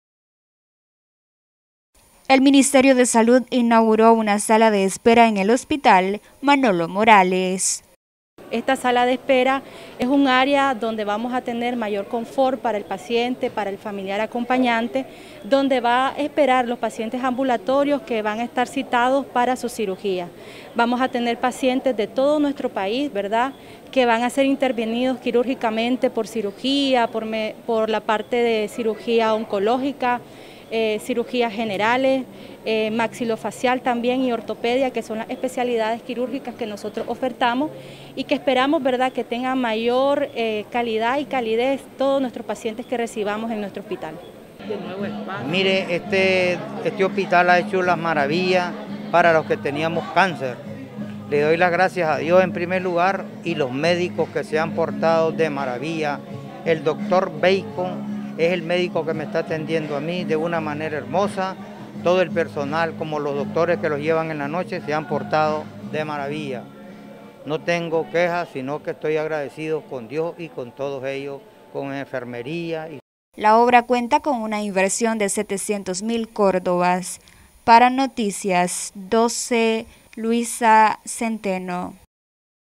Autoridades de salud de nicaragua inauguran sala de espera en hospital Manolo Morales
Noticias12Nicaragua Autoridades de salud de nicaragua inauguran sala de espera en hospital Manolo Morales